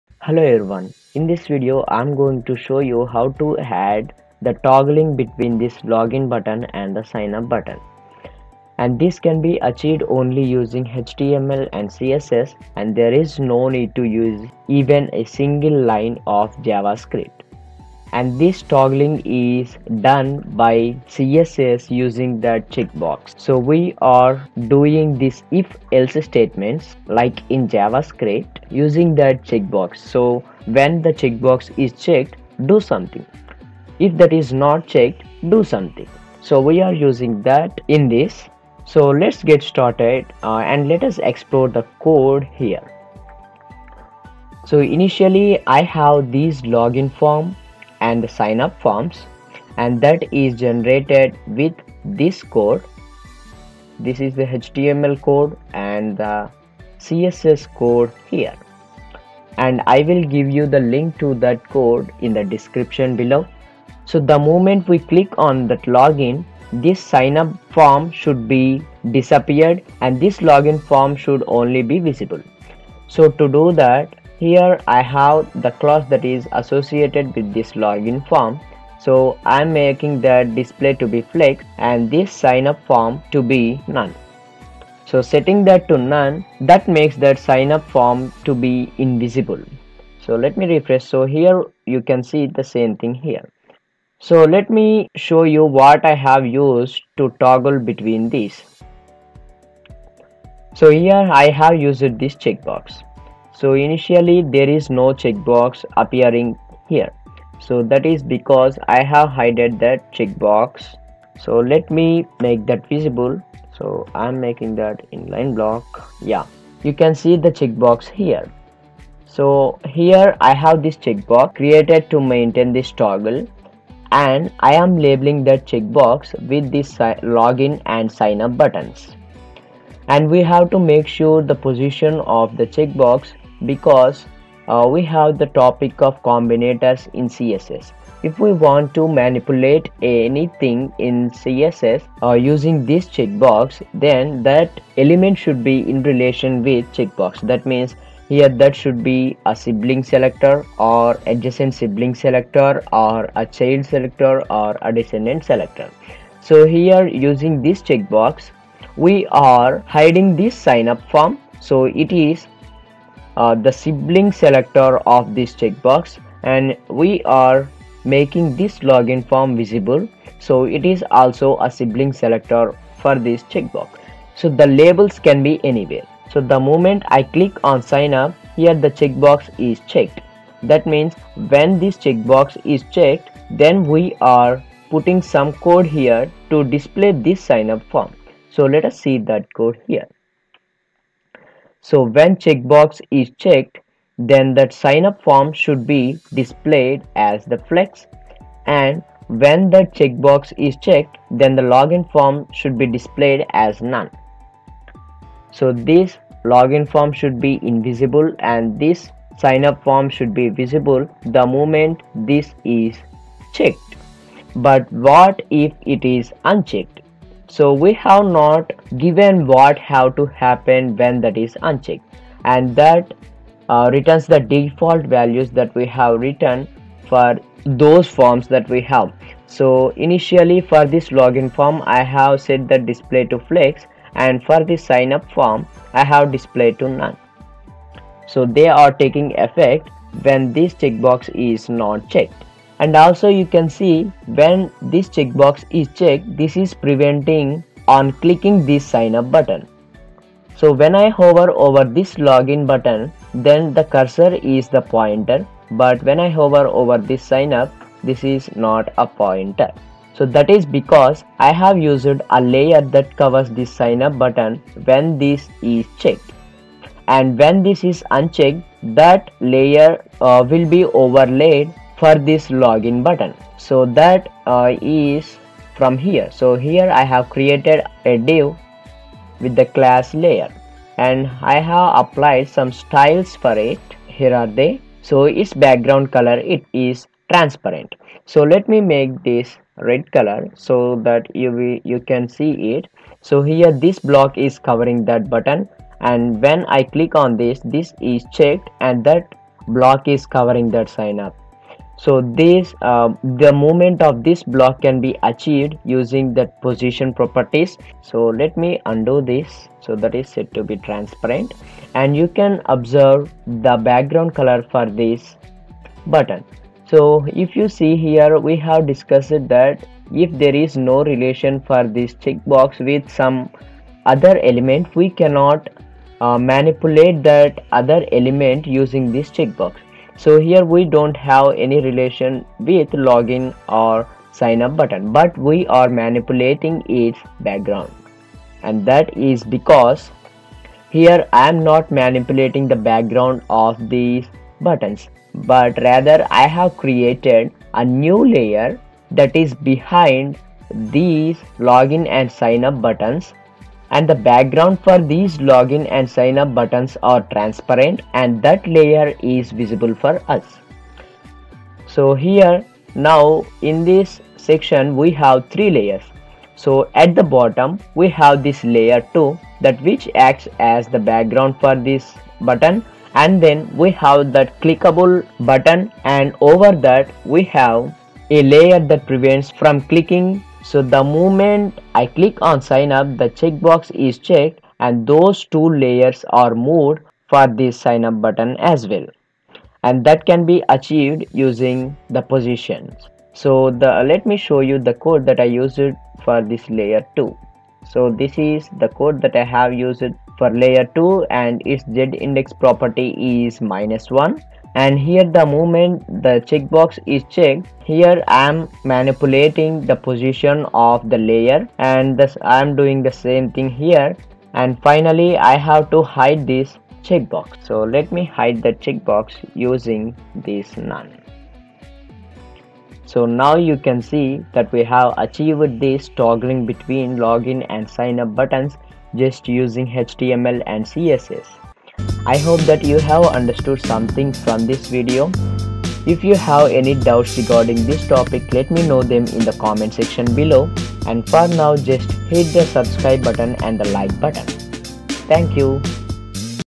Login and signup forms with a toggle option only using HTML and CSS || No Javascript || CSS if-else!
We can create login and signup forms with a toggle option only using HTML and CSS without javascript(No javascript). We use a checkbox to implement this toggle between those buttons.
Have you heard about CSS if-else statements? We can use checkboxes as if-else statements!

Login and signup forms with a toggle option only using HTML and CSS || No Javascript || CSS if-else!  -- Rate this video